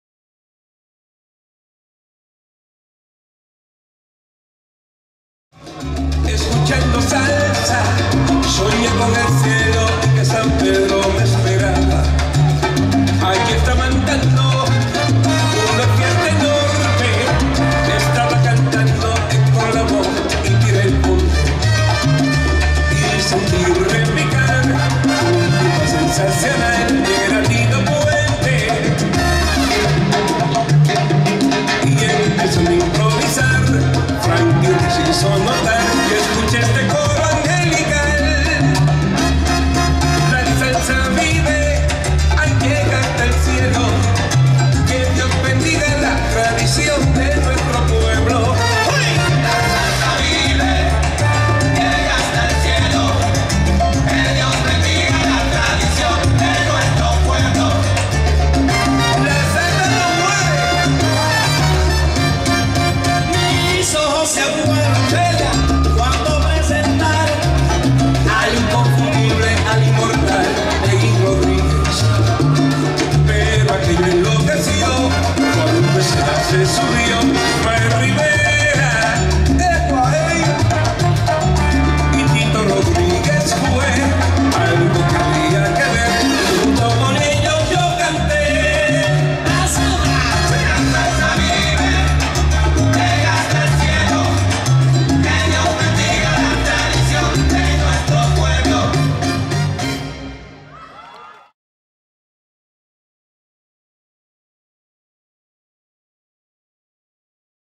MueBT2016 Toledo Campeonato de España Ciudad de Toledo Salsa de 18 a 30 D 10 11 12
I Final Autonómica de Baile Combinado de Castilla la Mancha
   Final Autonómica de Baile Combinado Comunidad de Madrid
I Trofeo Internacional de Baile Combinado ¨Ciudad de Toledo¨
Campeonato de España de Salsa ¨Ciudad de Toledo¨
Campeonato de España Bachata ¨Ciudad de Toledo¨
Campeonato de España de Chachachá ¨Ciudad de Toledo¨